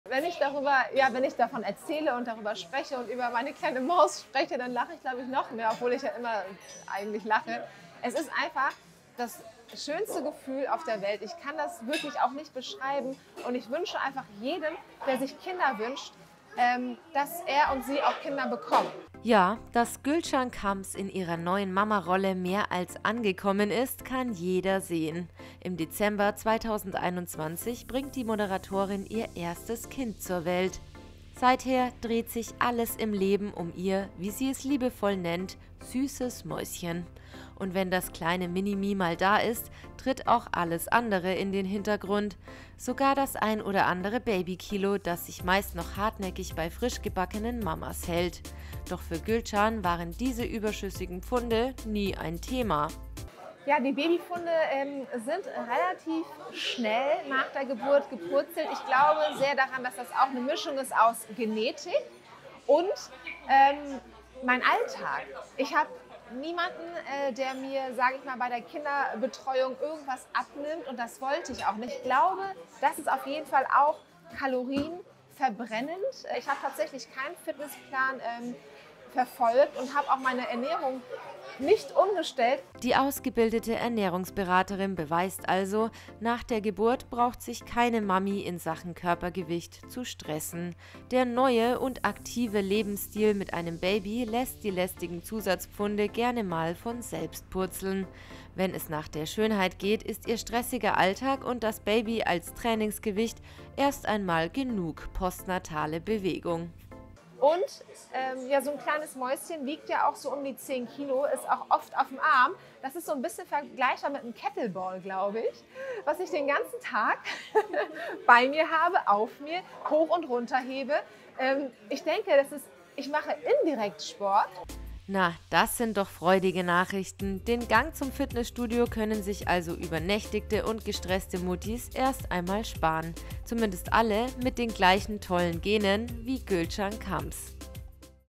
Gülcan Kamps – Im Interview: So purzelten ihre Babypfunde
Ja, dass Gülcan Kamps in ihrer neuen Mama-Rolle mehr als angekommen ist, kann jeder sehen. Im Dezember 2021 bringt die Moderatorin ihr erstes Kind zur Welt. Seither dreht sich alles im Leben um ihr, wie sie es liebevoll nennt “süßes Mäuschen“. Und wenn das kleine Mini-Me mal da ist, tritt auch alles andere in den Hintergrund. Sogar das ein oder andere Babykilo, das sich meist noch hartnäckig bei frisch gebackenen Mamas hält. Doch bei Gülcan waren diese überschüssigen Pfunde nie ein Thema.

Impressum:
Burda Forward
St.-Martin-Straße 66
81541 München